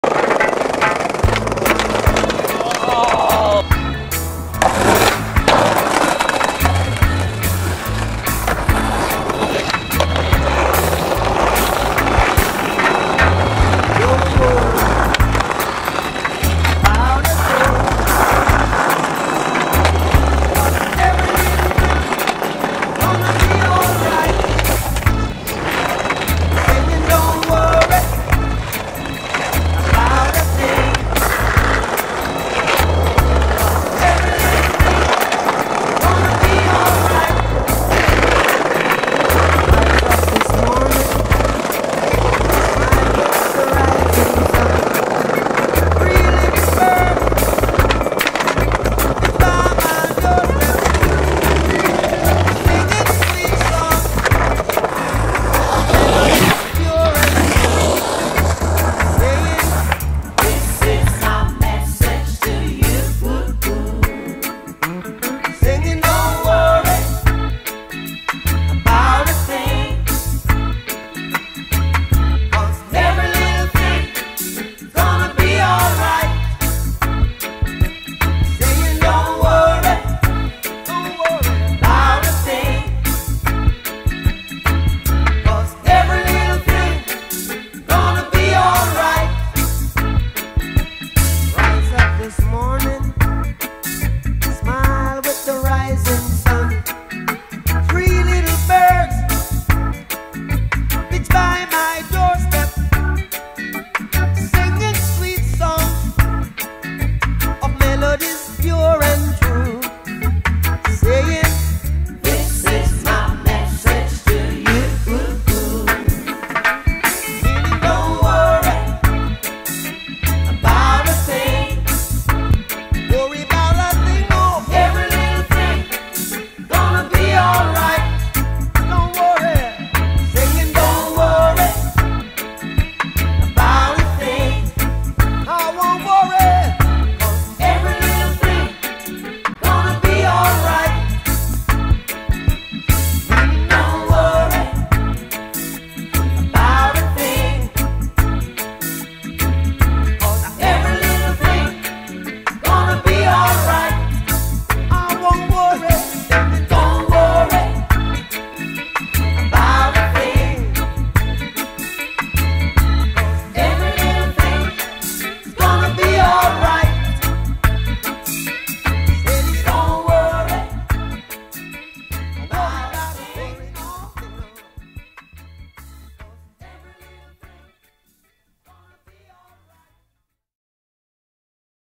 natt weston boston.mov
messing around skating in downtown boston
i dont know why the whole song stayed on my computers gay